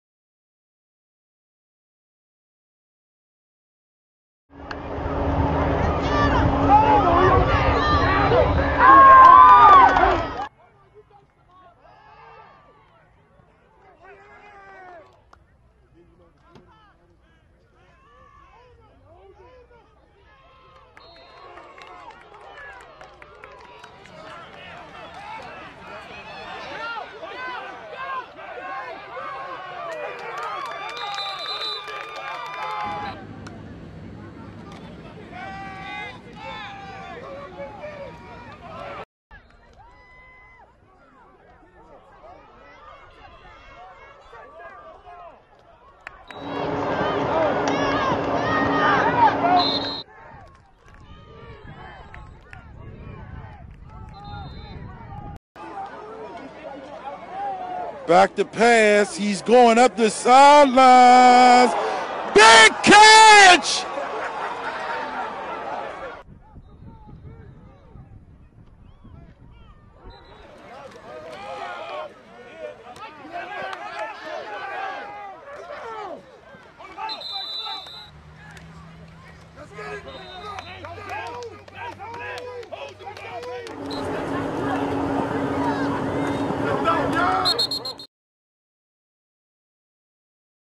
2025 QB/ATH • Denzel Garner • 5’11” 175 lbs • Dematha High School
8th Grade Highlights The Future is Bright